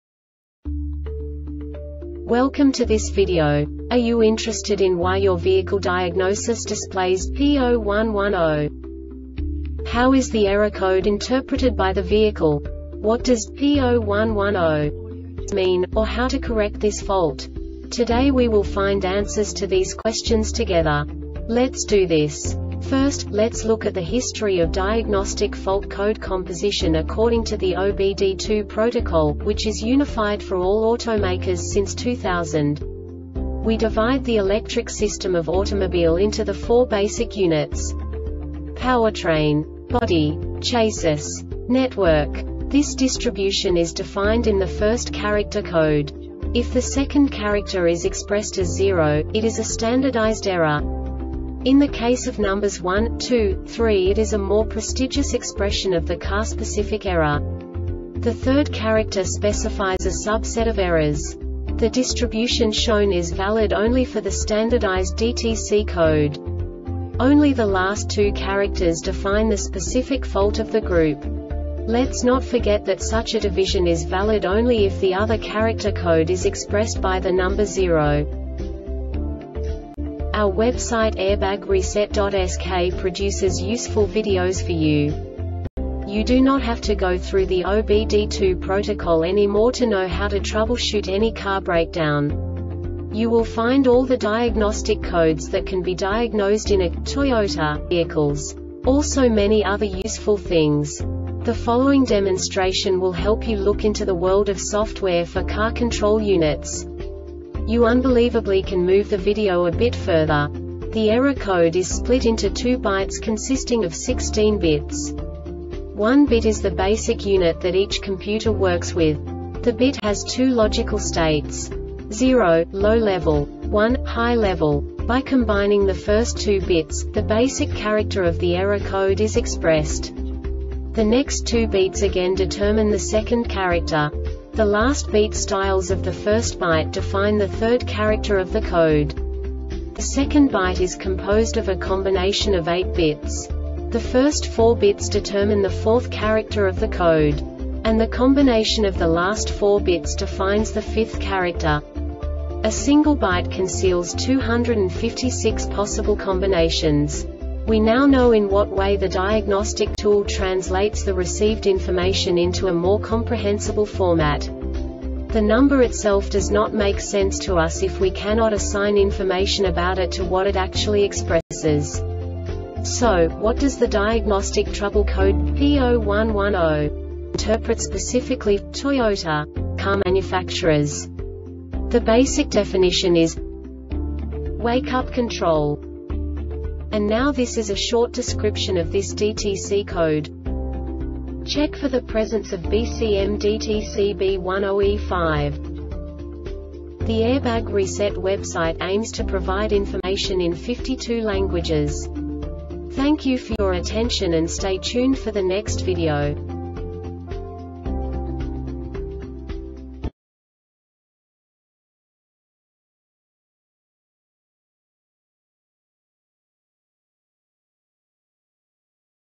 DTC Toyota P0110 Short Explanation
Contents:
0:21 Basic DTC analysis according to OBD2 protocol standard.
1:48 Insight into programming
2:58 A diagnostically understandable description of the Diagnostic Trouble Code
3:05 Basic error definition

Make:
Toyota
Code:
P0110
Definition:
Wakeup Control
Diagnosis: